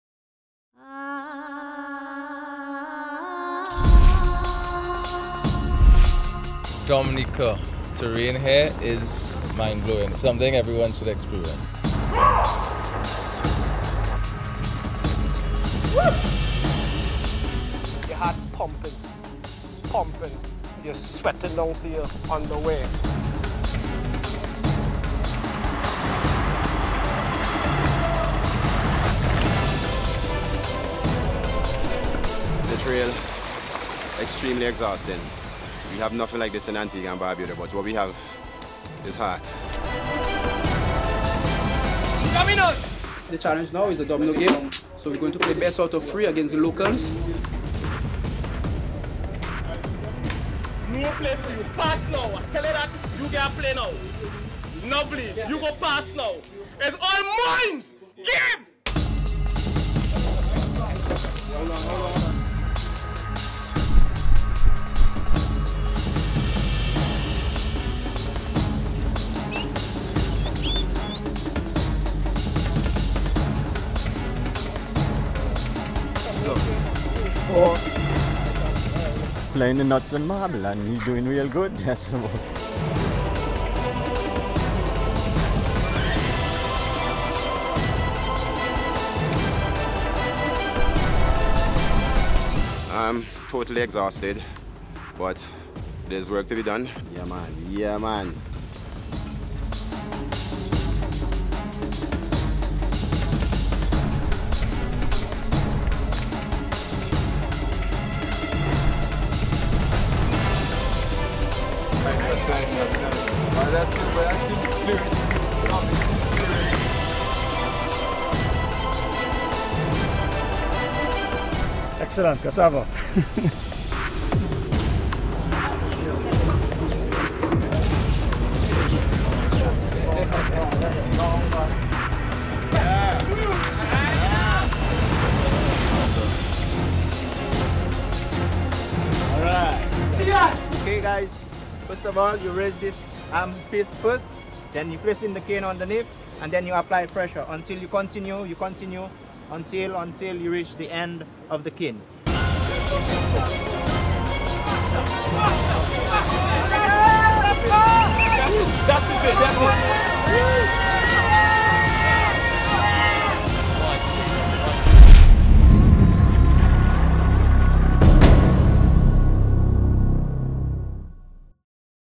Nature Island Challenge - Day 3 Highlights
While the weather improved for Day 3 the trail was just as tough as teams raced to the Carib Territory!